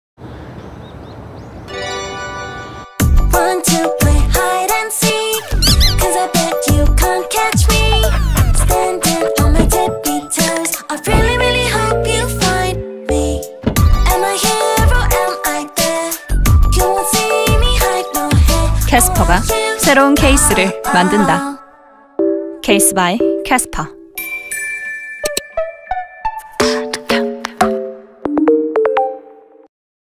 광고 캐스퍼 프리런칭 광고 '새로운 케이스' 편 CASPER Pre Launching, 30s
다들 저 기다리셨죠? 😎

캐스퍼가 새로운 케이스를 만든다.
case by CASPER, 지금 사전 클릭 중!

여러분은 어떤 case인가요?
가장 먼저 캐스퍼를 경험하고 싶다면
지금 얼리버드 예약하세요. 🏃‍♂🏃‍♀🏃